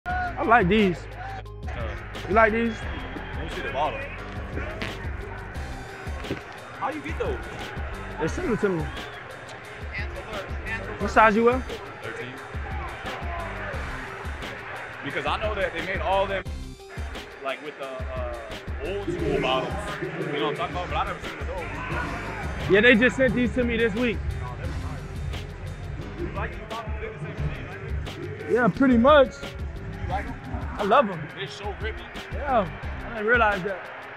Jalen Hurts and Dallas Goedert React to Their NEW Cleats!
Watch as Philadelphia Eagles quarterback Jalen Hurts and tight end Dallas Goedert react to their new cleats at the matchup against the Pittsburgh Steelers.